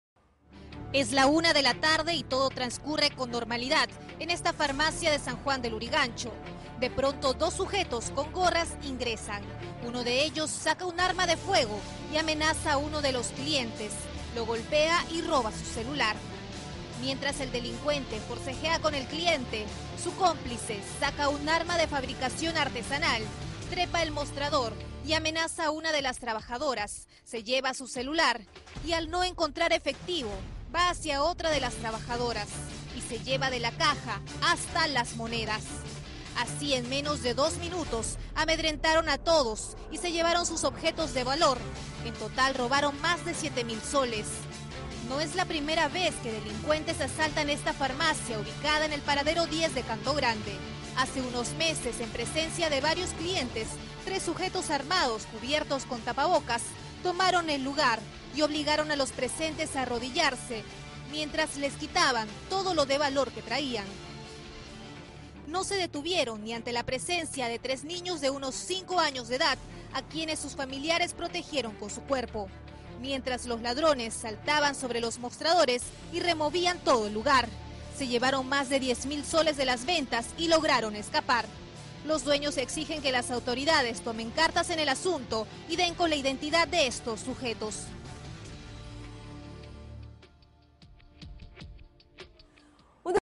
SJL: asaltan farmacia en menos de dos minutos y se llevan siete mil soles
En el distrito de San Juan de Lurigancho dos ladrones ingresaron a una farmacia fingiendo ser clientes y asaltaron a trabajadores y clientes. Los sujetos sacaron armas de fuego y mientras uno despojaba de sus pertenencias a los consumidores, su cómplice trepaba el mostrador y amenazaba a las vendedoras.

Así se llevaron efectivo, monedas y objetos de valor; en menos de dos minutos consiguieron huir con siete mil soles de las ganancias del negocio ubicado en el paradero 10 de Cantogrande.

Hace unos meses en este mismo establecimiento tres sujetos armados y encapuchados perpetraron el mismo delito, aquella vez se llevaron 10 mil soles. Los dueños del local piden incrementar la seguridad en la zona.